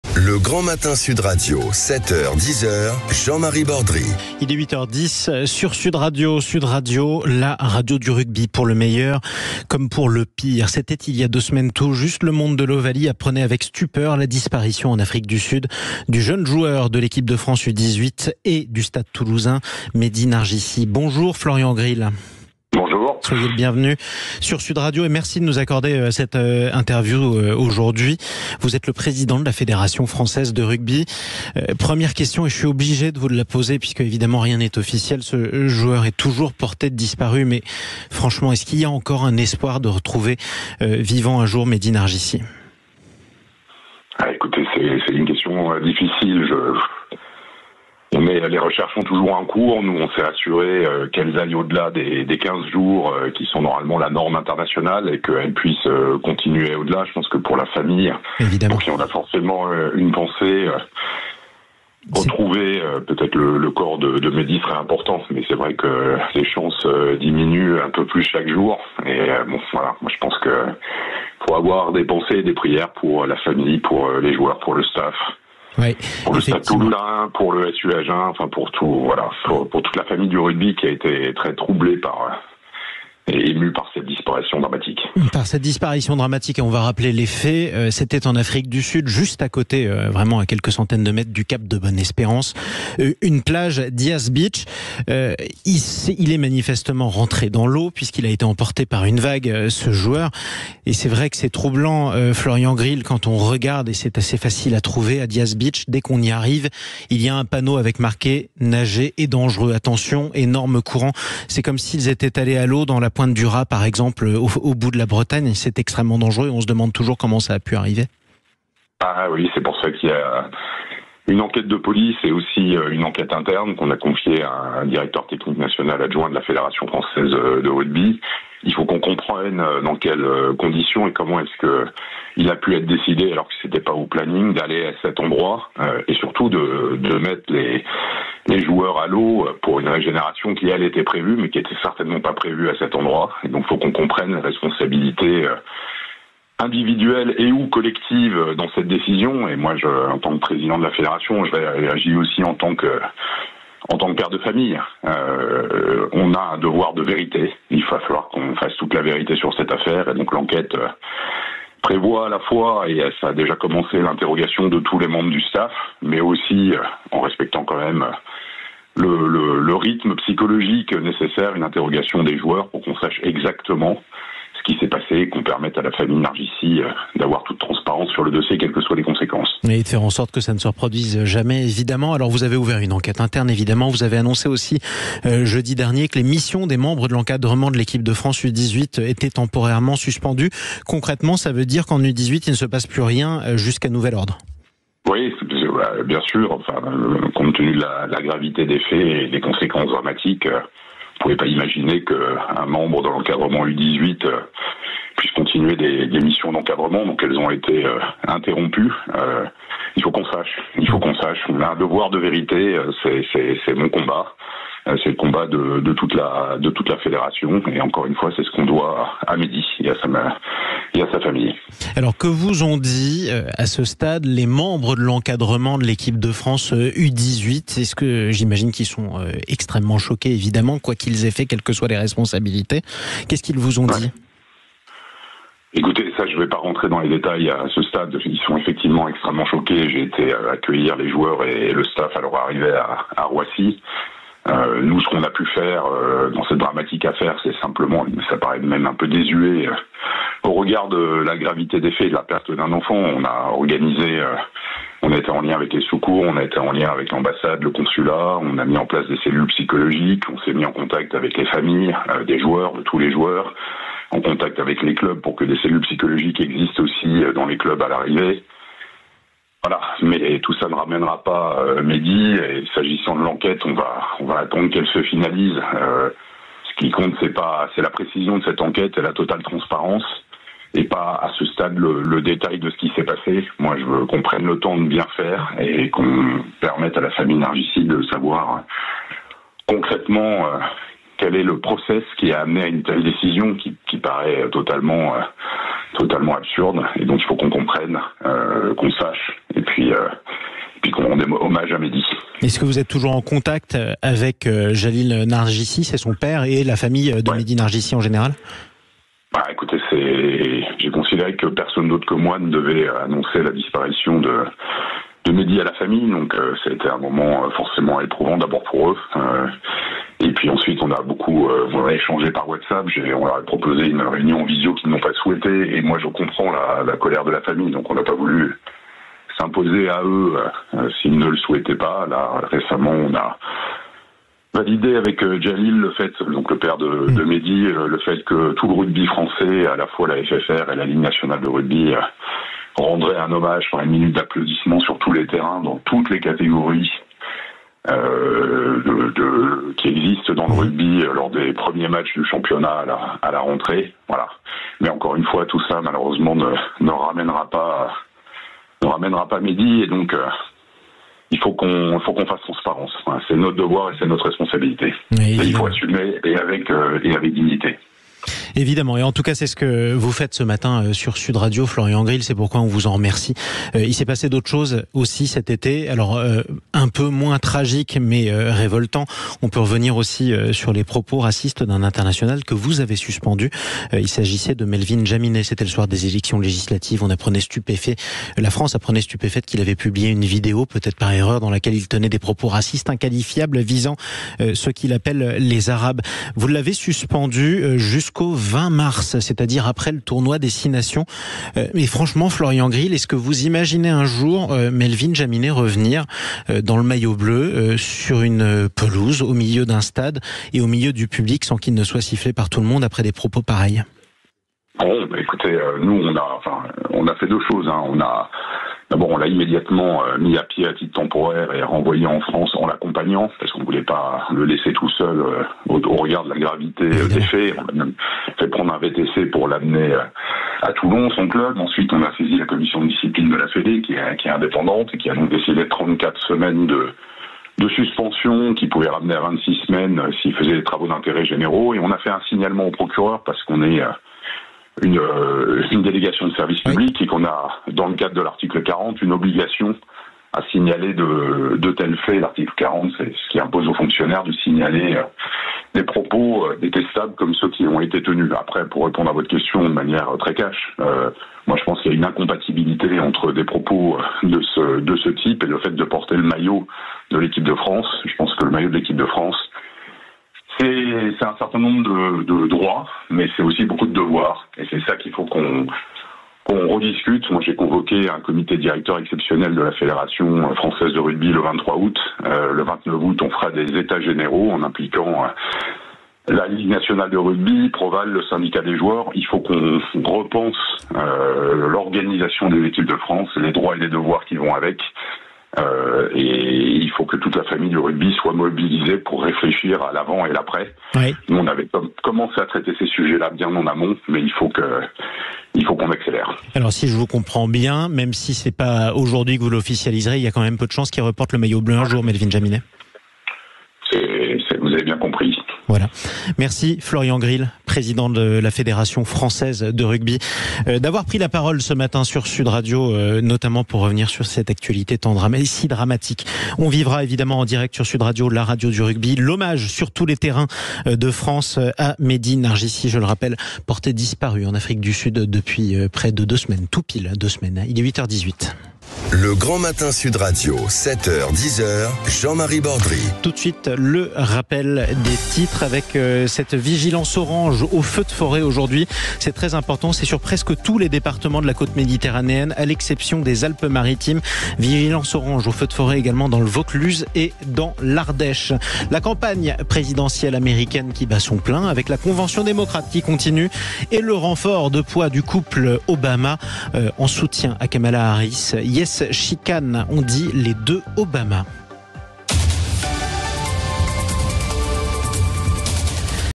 Disparition de Mehdi Narjissi, 15 jours après : la réaction de Florian Grill
Avec Florian Grill, Président de la Fédération Française de Rugby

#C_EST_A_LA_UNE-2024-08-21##